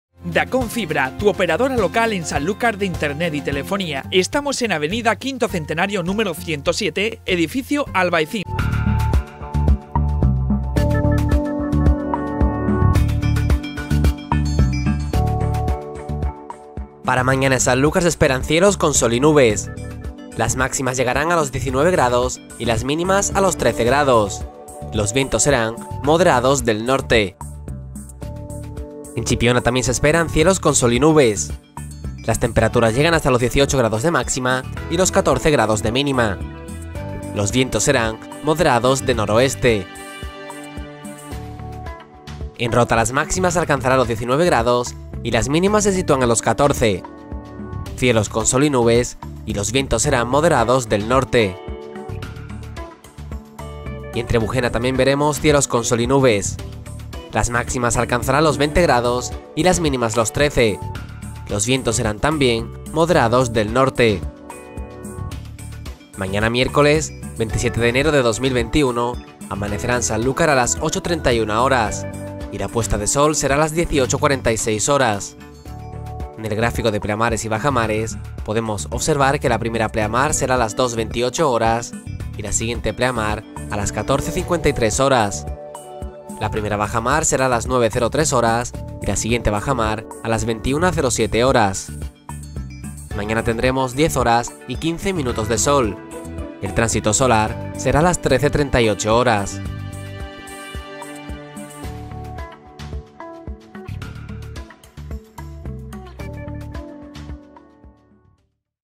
Pronóstico del tiempo para el miércoles día 27 de enero de 2021 en Sanlúcar
Pronóstico del tiempo para el miércoles día 27 de enero de 2021 en Sanlúcar de Barrameda y la Costa Noroeste.